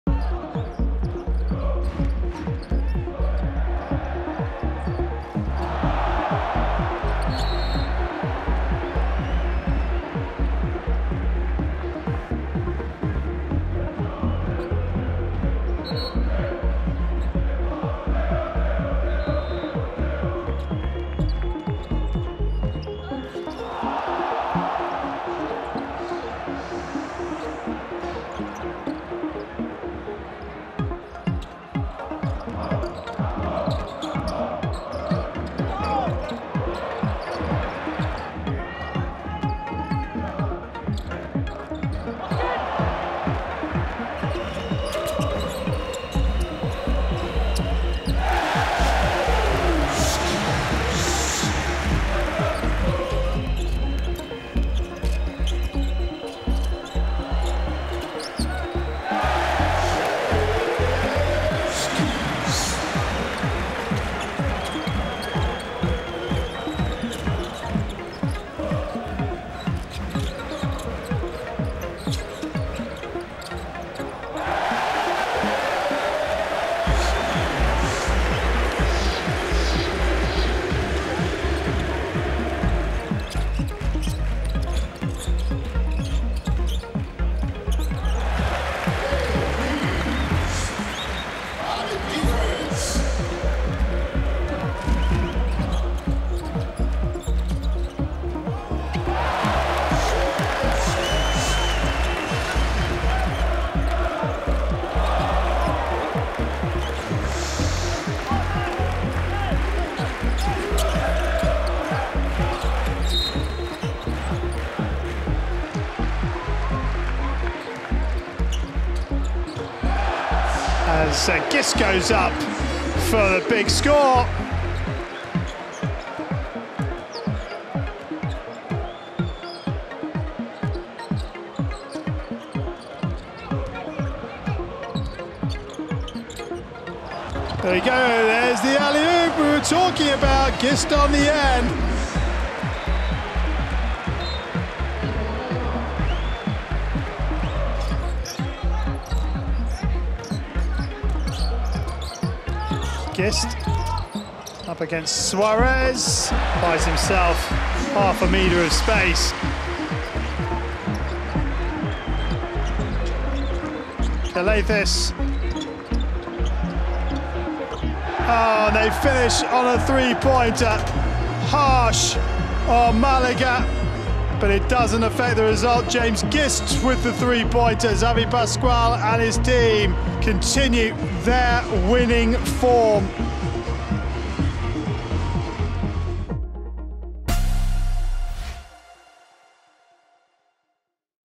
From the archive: James Gist highlights
Among the many superstars who have graced Turkish Airlines EuroLeague courts over the last two decades, James Gist has given fans plenty of special moments to enjoy. Check out some of his best!